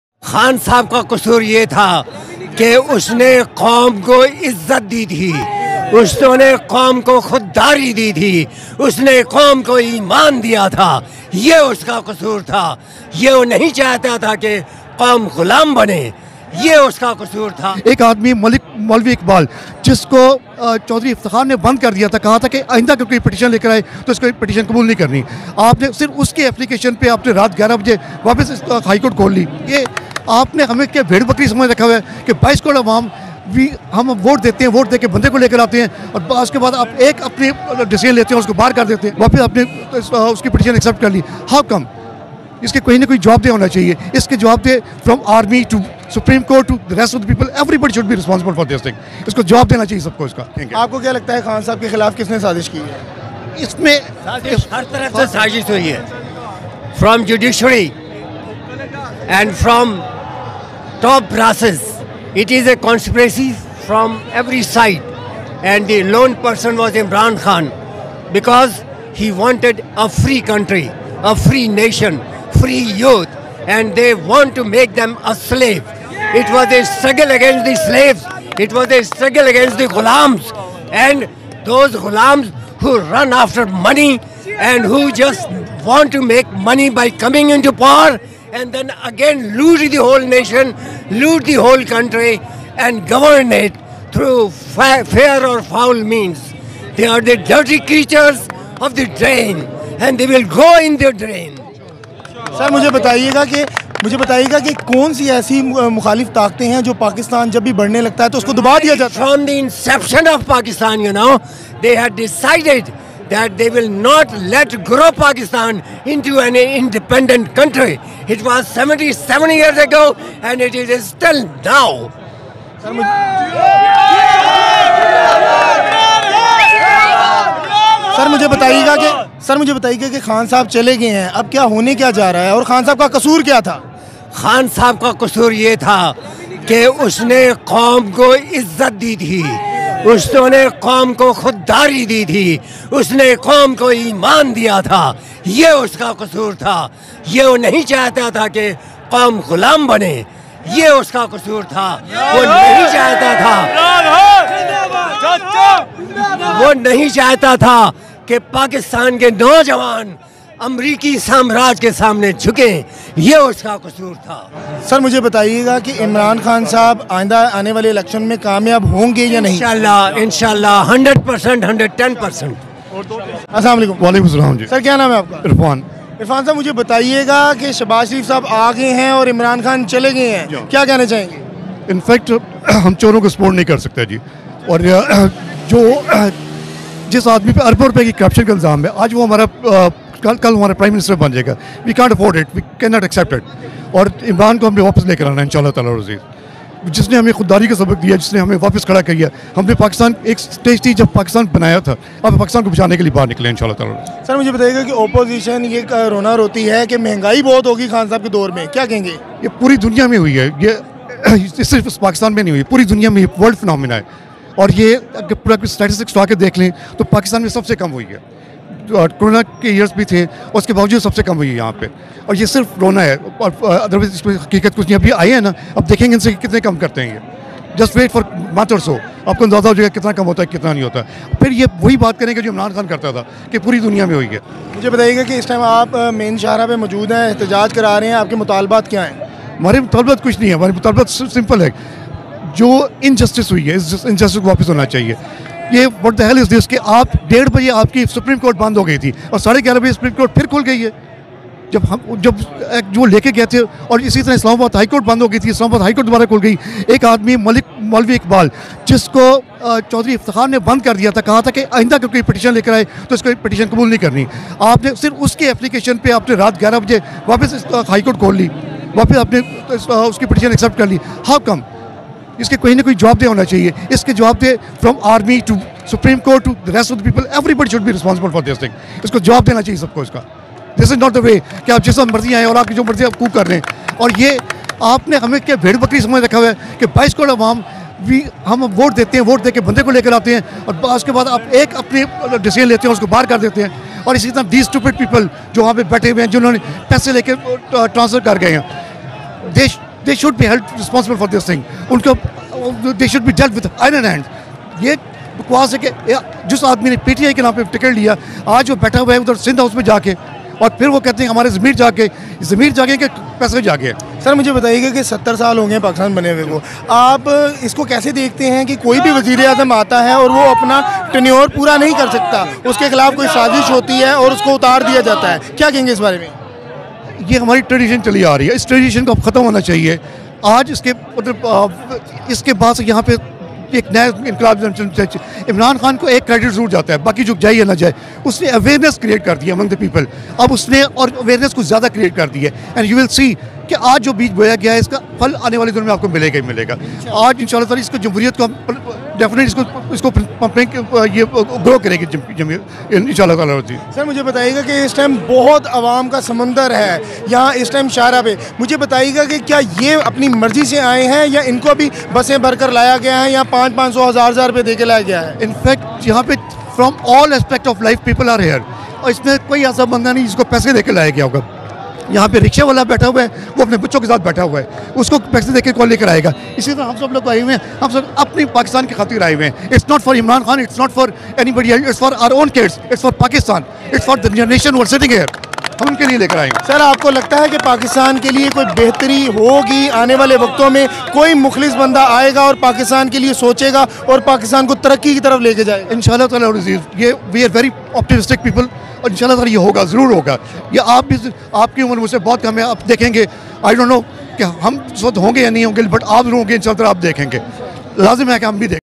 Imran Khan Ki Call Par Mulk Bhar Main Ehtijaj | Sabiq Wazir-e-Azam Ki Janib Se Awam Ka Shukriya
Host: Mian Abdullah
Camera & Direct : Ameen Jutt

Thanks!